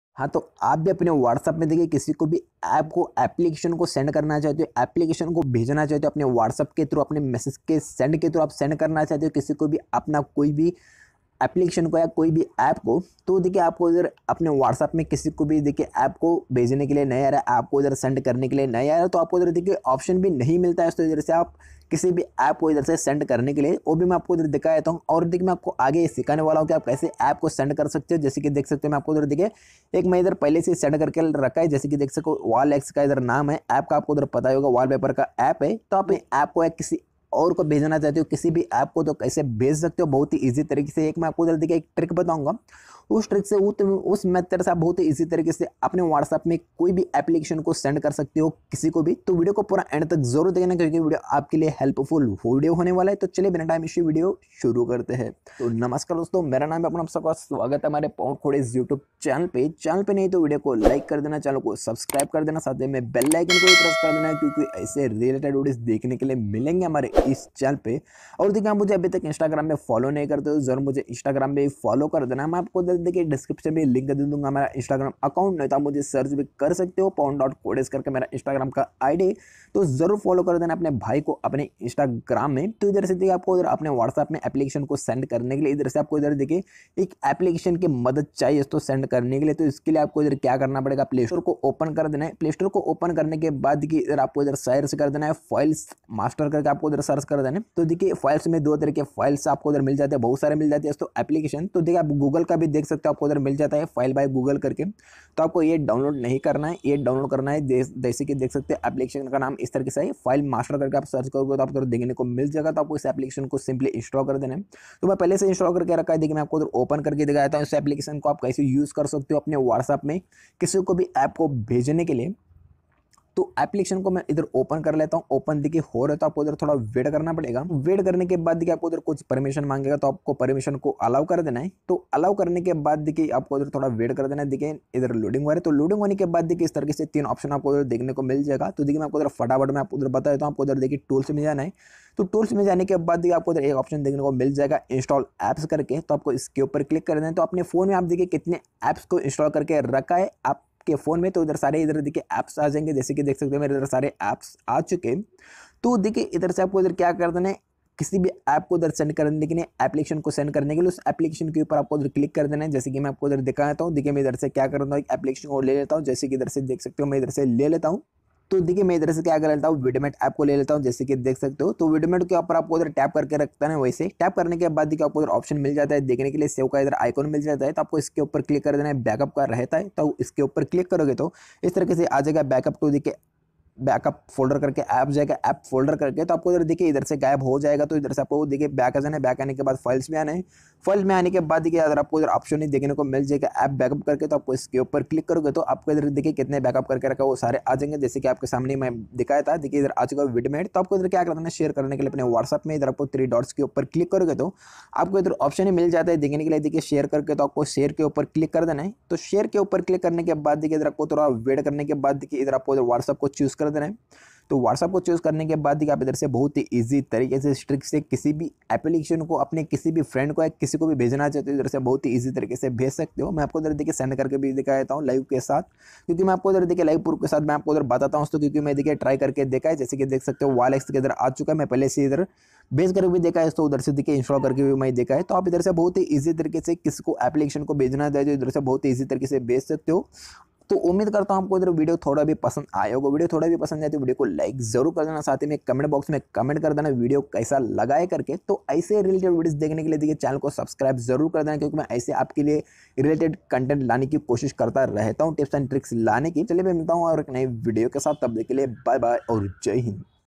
Whatsapp Par App Kaise Send Karen | How To Send Apps And Games On Whatsapp | Share Apps On Whatsapp
Dosto iss video me aap sikh jayenge ki apne whatsapp me kisi ko bhi koi bhi app ko kaise bheje, whatsapp se apps ko send kaise kare to aap bhi kisi ko whatsapp se application ko send karna cha rahe ho lekin aap ko whatsapp se application ko send karne ke liye nahi araha hai to ye video aap ke liye hai to umeed karta hu aap ko ye video pasand aayega